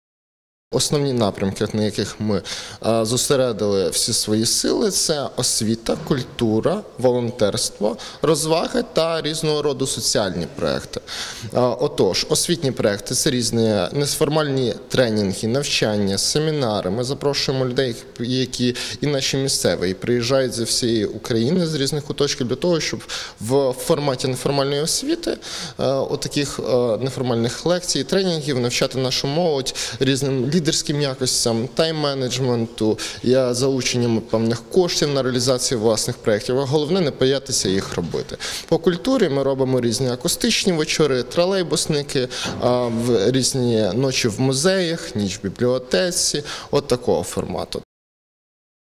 «Молодь за краще майбутнє» шукає активних і цілеспрямованих! (1)
Ти молодий , амбітний і повний ідей?- Тоді хмельницька організація «Молодь за краще майбутнє» пропонує приєднатися до своїх лав. Переконують : підкажуть, як реалізувати «старт-апи», познайомлять з успішними та цікавими та просто підкажуть корисну інформацію.